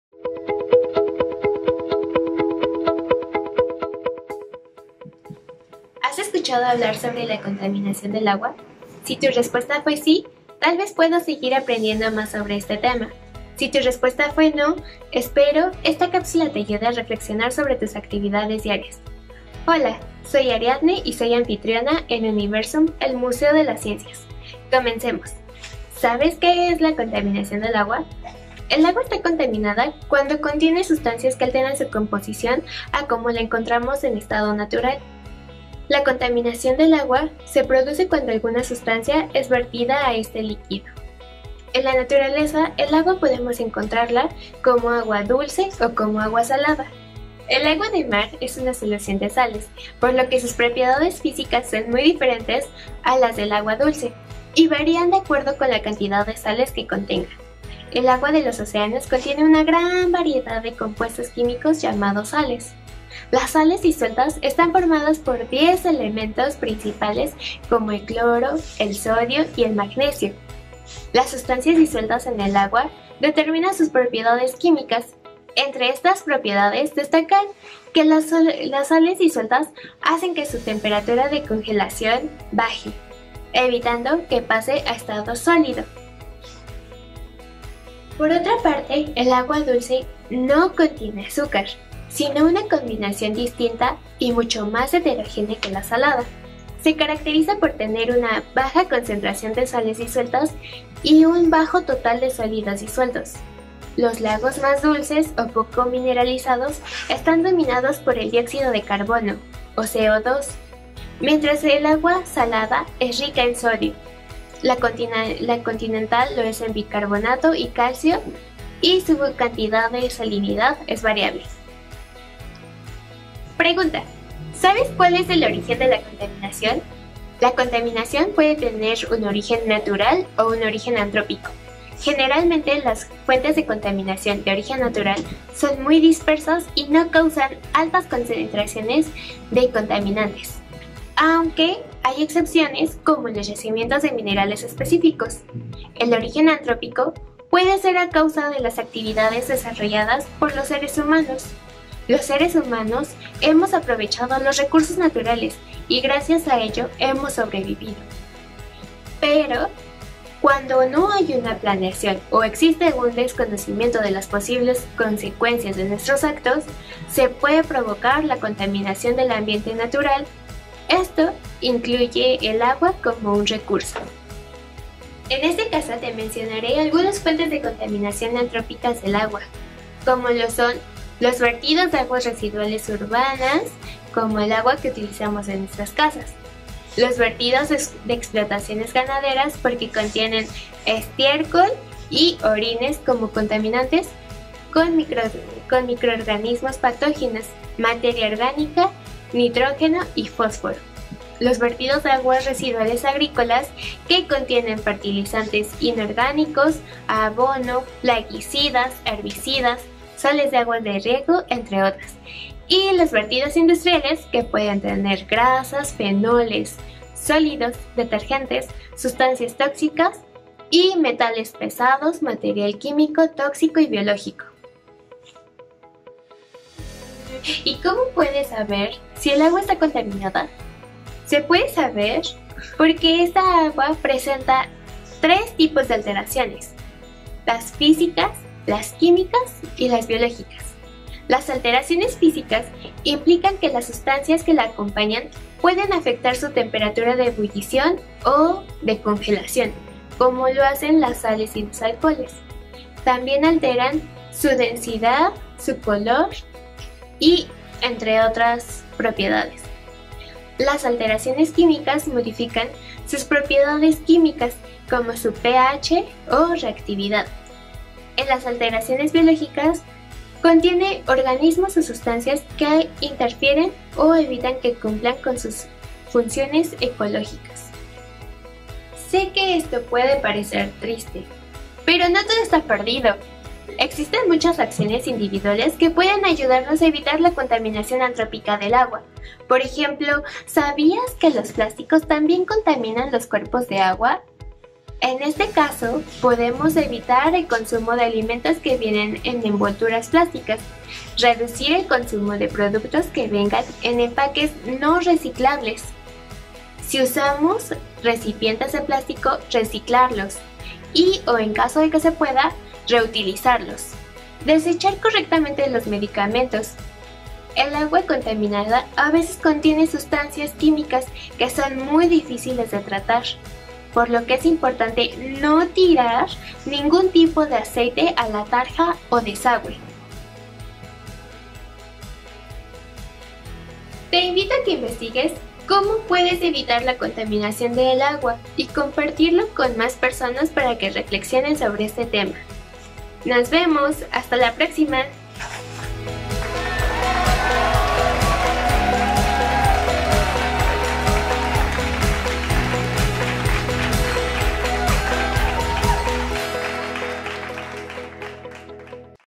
Contaminación del agua
¿Cuántas son las maneras en las que contaminamos el agua? ¿Cómo la química se usa para purificarla?

¡Sigue este video de AnfitrionesUniversum!